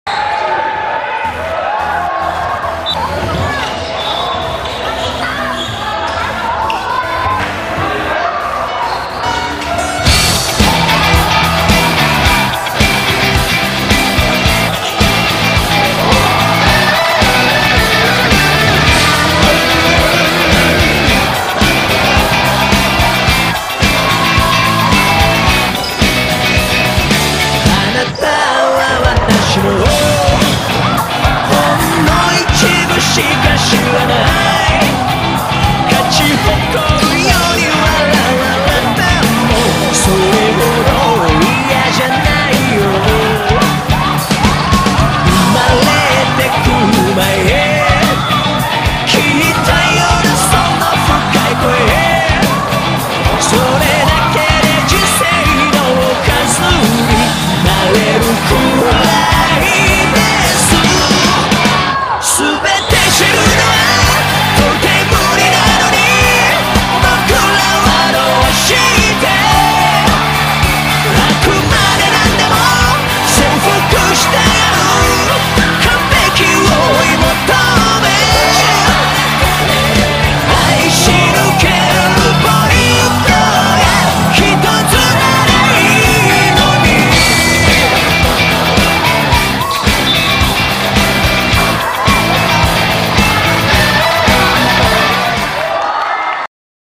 李兆基籃球丙組10號(2017.5.5.)槍林彈雨特輯
2017.5.5.天瑞體育館 17:10
元朗學界男子籃球丙組決賽
李兆基(白衫)10號